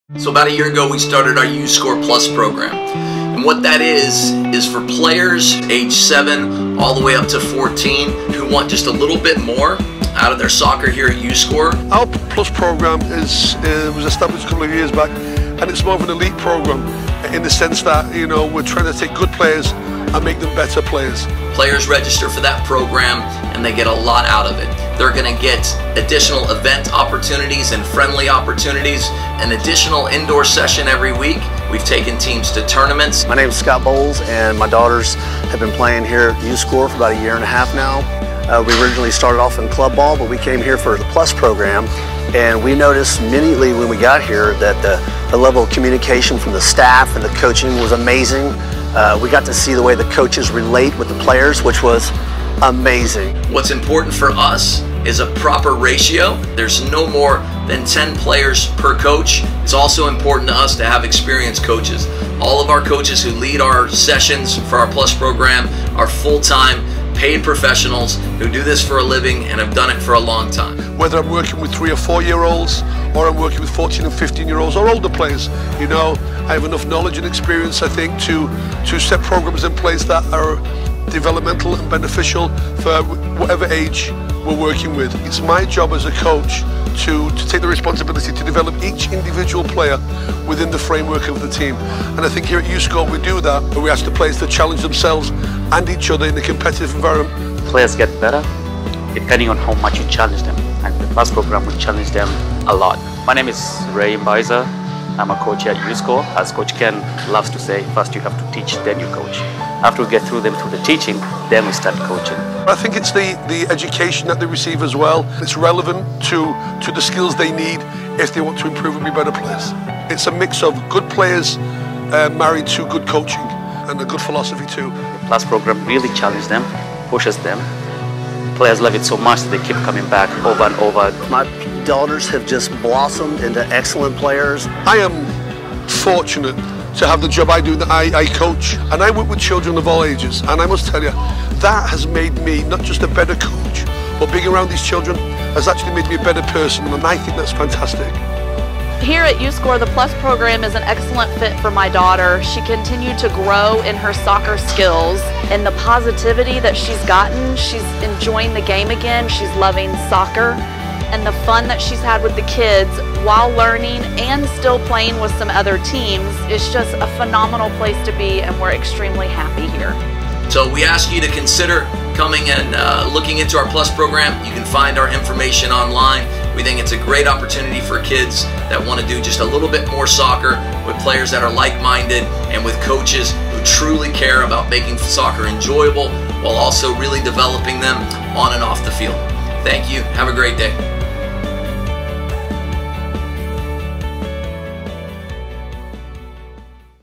uScore Plus Program
The uScore Plus Program is a higher level training platform for those who want just a little bit more out of the uScore soccer experience.

The Plus Program is designed to elevate players, aged 7 - 14, through additional event opportunities, an additional indoor session every week, and more.

Interested in learning more or signing your child up?